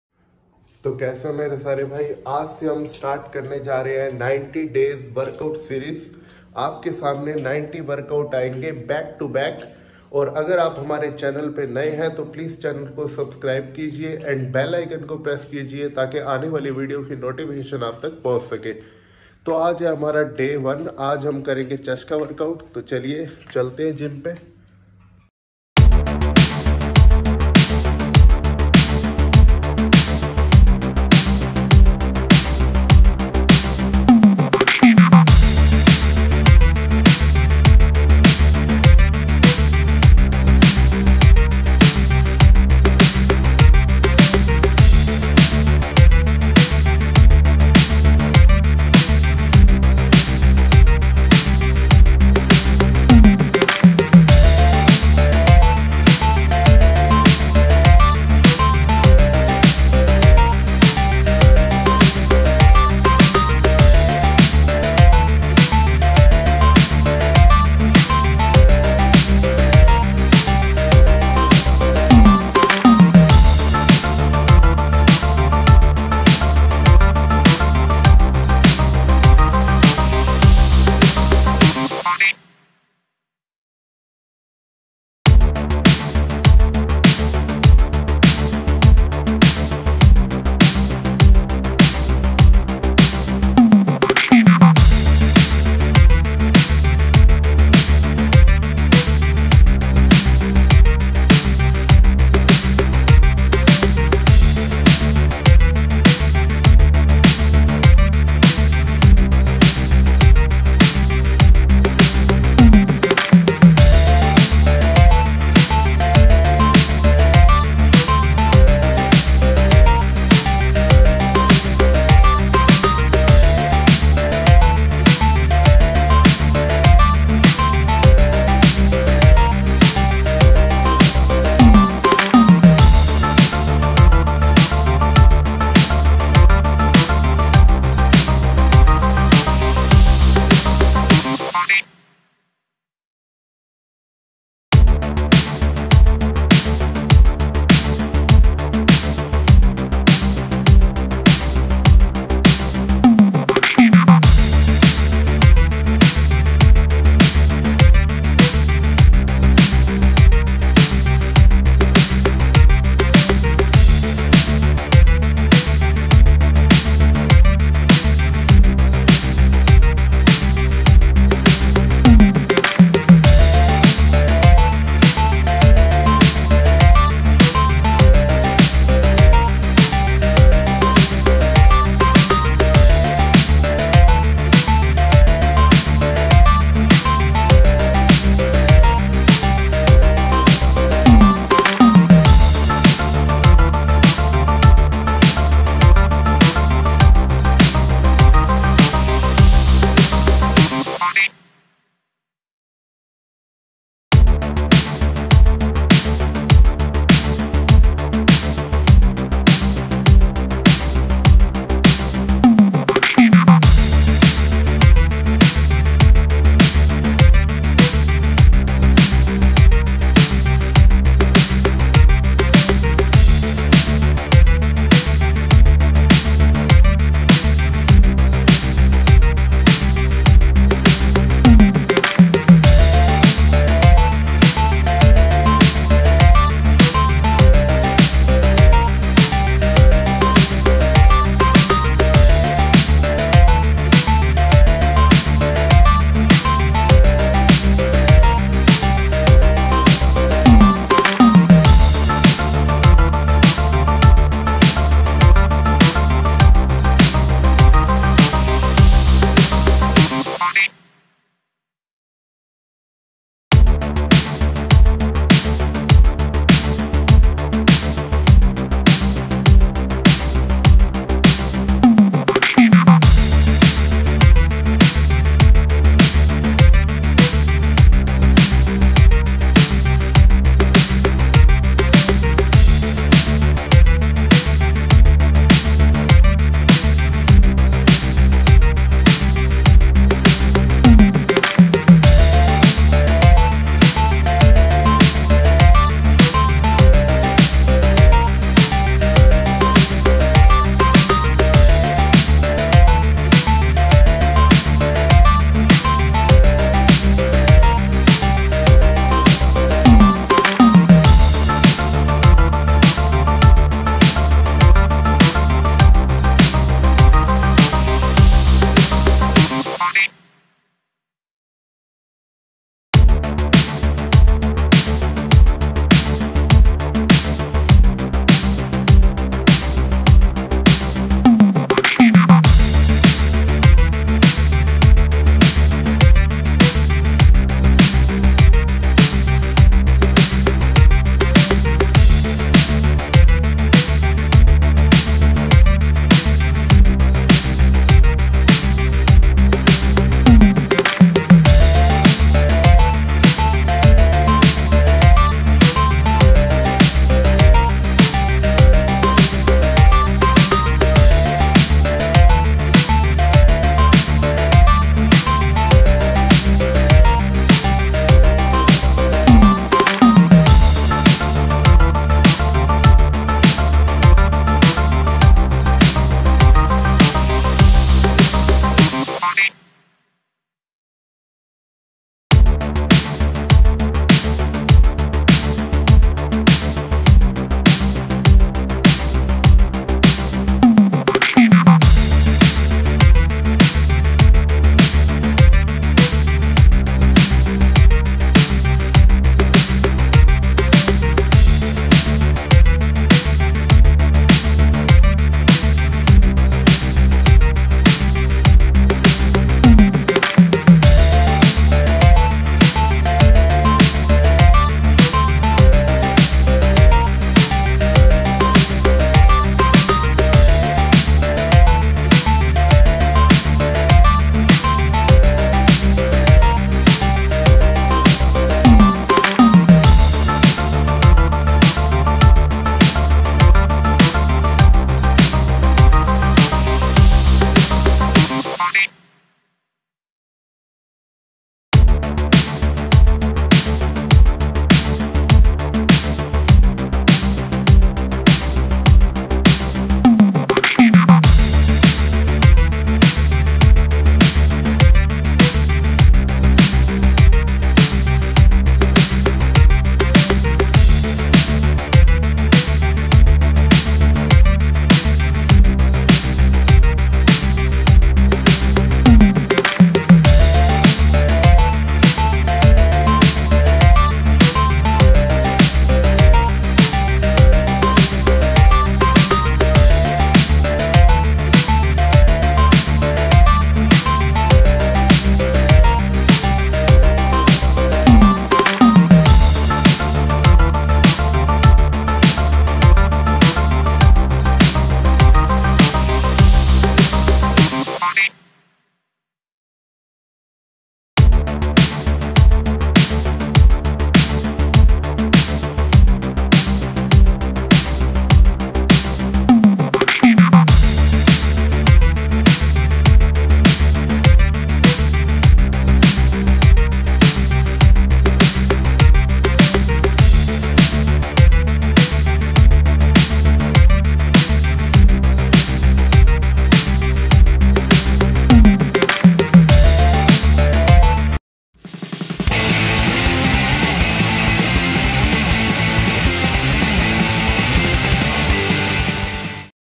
90 DAYS WORKOUT SERIES DAY 1 CHEST WORKOUT#chest#gym#bodybuilding#fitnesslifestyle#motivation
is vdo me maine chest wrkout k bare me btaya h